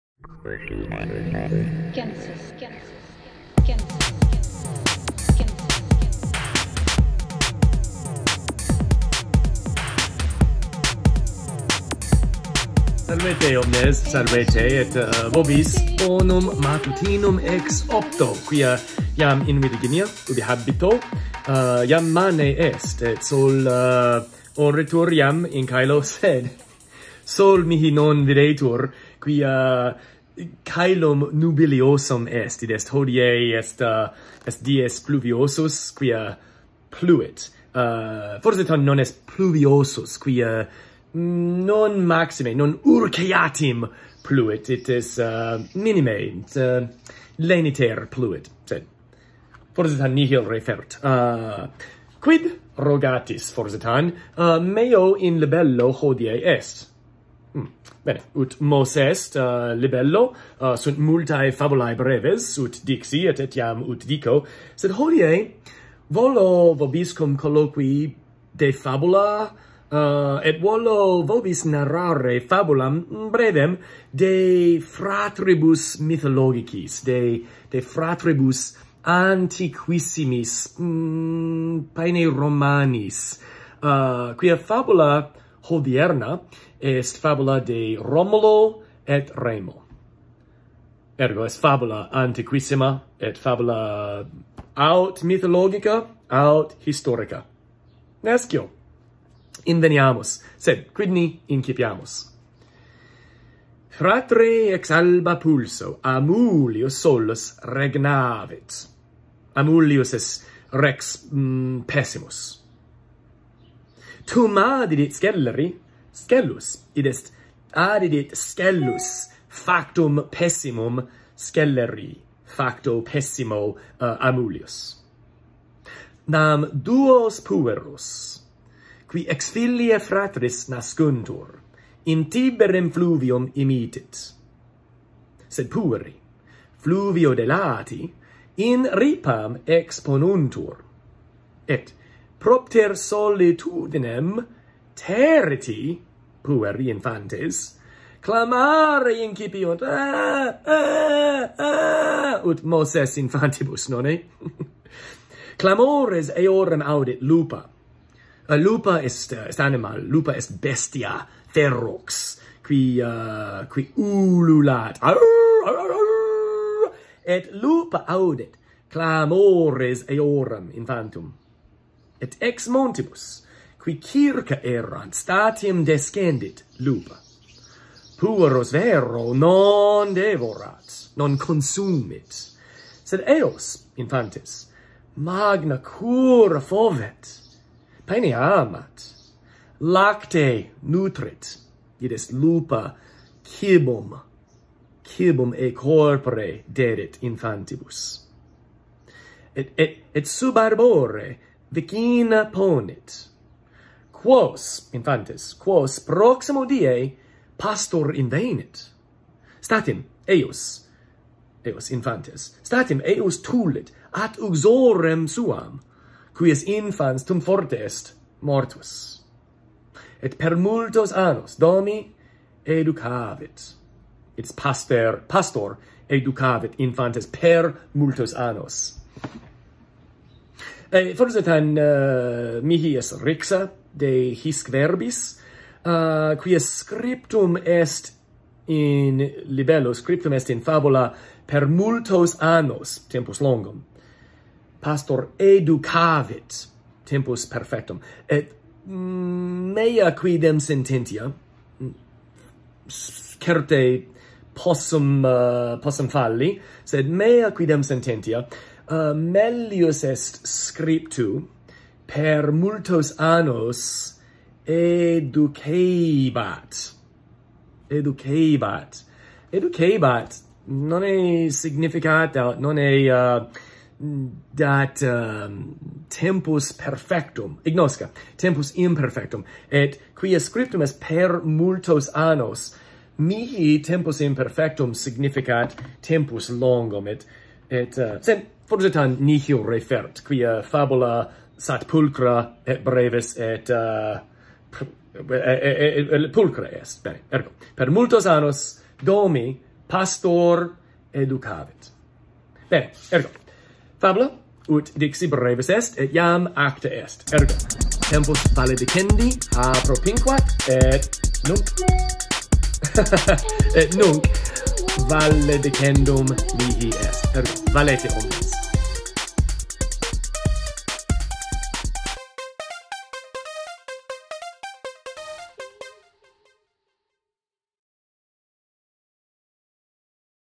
De Fratribus Mythologicis
Hāc in pelliculā fabulam de fratribus mythologicis -- aut historicis? -- lego et de fabulā et vocabulā breve vobiscum colloqui volo. Fruamini!